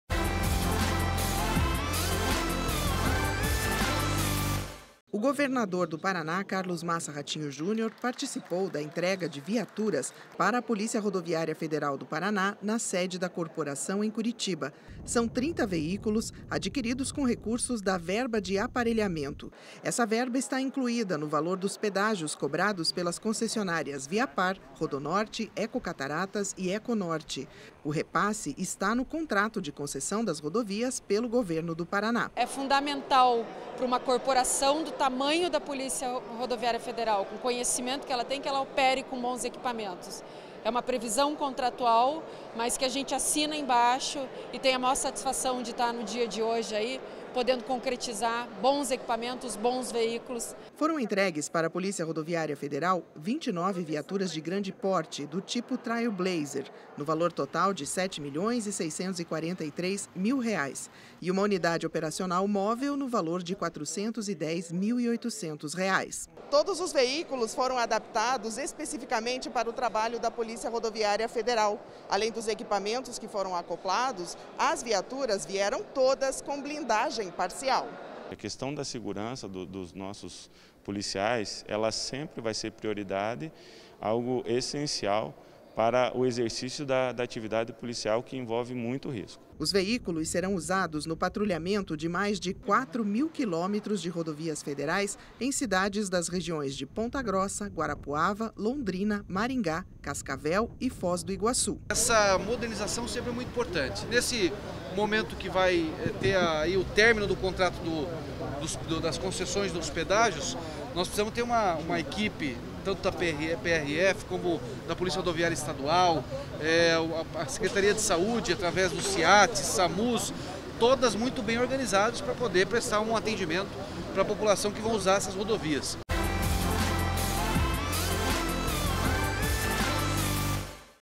Polícia Rodoviária Federal do Paraná ganha 30 novas viaturas
As novas viaturas vão ampliar e melhorar o monitoramento das rodovias federais que cortam o Estado. O investimento é de R$ 8 milhões e refere-se ao repasse contratual das concessionárias que administram os trechos rodoviários, oriundo das tarifas de pedágio.